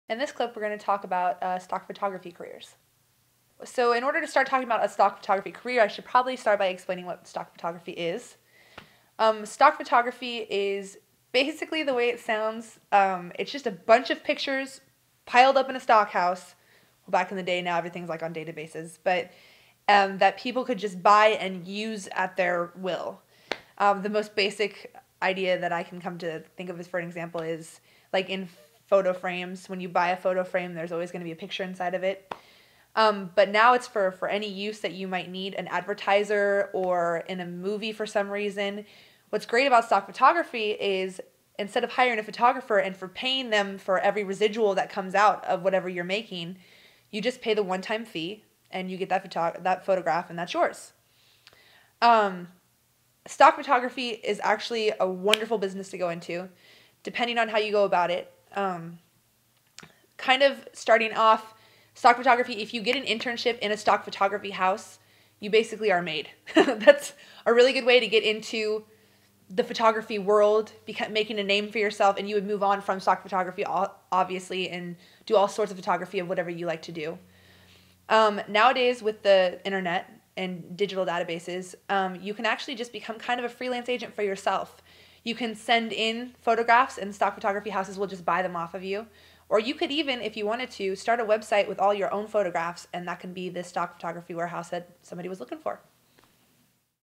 Photography Tips & Projects : About Stock Photography Careers
Stock photography careers can be pretty lucrative, as there is a large market for it, and it is easy to submit digital photos to stock photography houses. Start a freelance career in stock photography with information from a professional freelance photographer in this free video on photography.

Expert: Lauryn Spielvogel
Bio: Lauryn Spielvogel is a professional freelance photographer with a wide range of commercial and artistic experience.
Filmmaker: Nathan Boehme